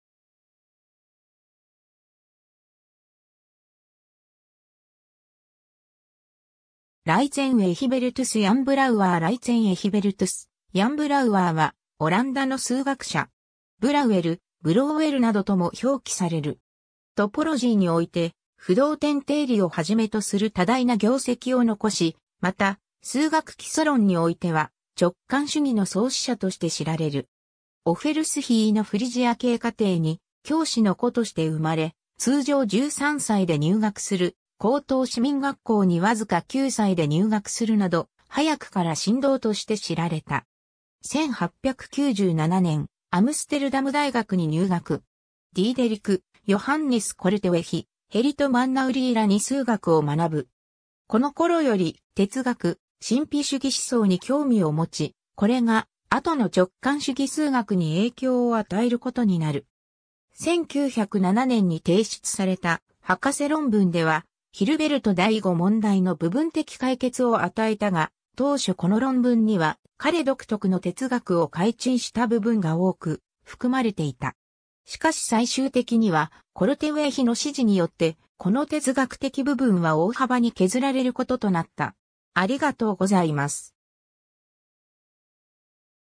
ライツェン・エヒベルトゥス・ヤン・ブラウワー
ライツェン・エヒベルトゥス・ヤン・ブラウワー ライツェン・エヒベルトゥス・ヤン・ブラウワー（Luitzen Egbertus Jan Brouwer、1881年2月27日 - 1966年12月2日）はオランダの数学者。
ブラウエル、ブローウェルなどとも表記される。
トポロジーにおいて不動点定理をはじめとする多大な業績を残し、また数学基礎論においては直観主義 (数学の哲学)の創始者として知られる。
 オフェルスヒー（現在はロッテルダムの一部）のフリジア系家庭に教師の子として生まれ、通常13歳で入学する高等市民学校にわずか9歳（当時のオランダ最年少記録）で入学するなど、早くから神童として知られた。
 1897年、アムステルダム大学に入学。
ディーデリク・ヨハンネス・コルテウェヒ（KdV方程式で有名）、ヘリト・マンナウリーらに数学を学ぶ。
このころより哲学・神秘主義思想に興味を持ち、これが後の直観主義数学に影響を与えることになる。
 1907年に提出された博士論文ではヒルベルト第5問題の部分的解決を与えたが、当初この論文には、彼独特の哲学を開陳した部分が多く含まれていた。
しかし最終的にはコルテウェヒの指示によって、この哲学的部分は大幅に削られることとなった。